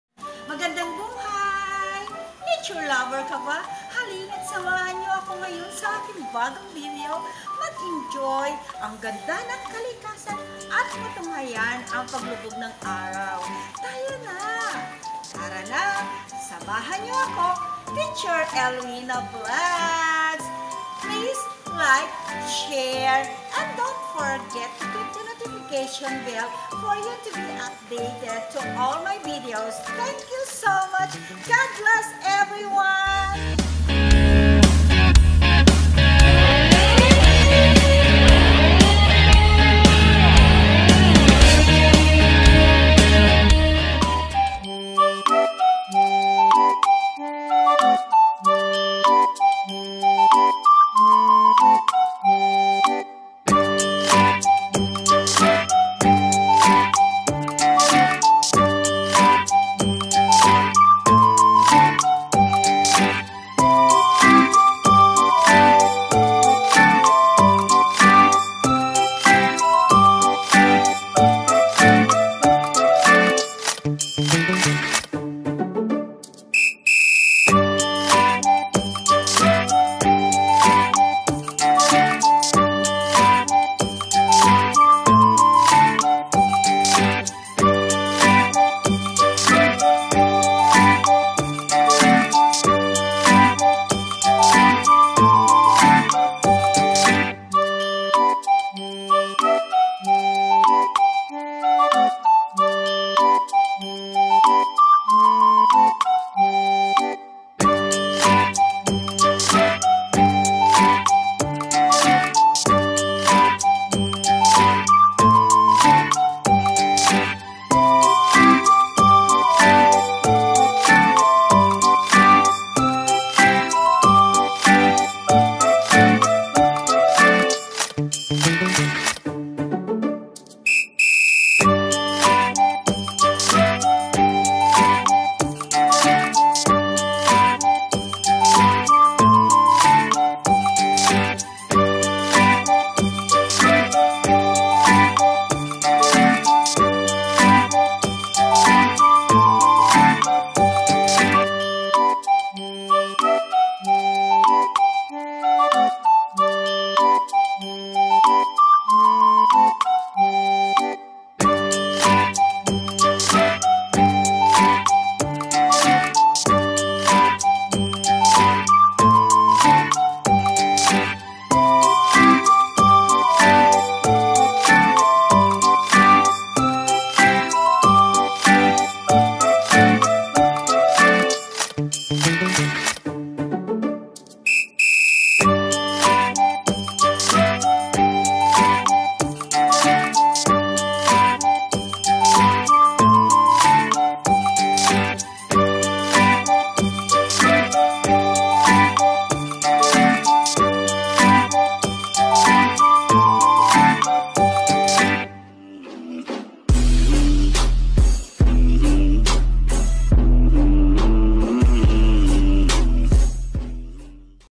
Nature Lover ka ba?? Tara Samahan Niyo Ako | Teacher Elwina Vlogs
Video Taken at Brgy. Lewin, Lumban Laguna @CalirayaResortClub

FOR NOTIFICATION OF MY NEW VIDEO !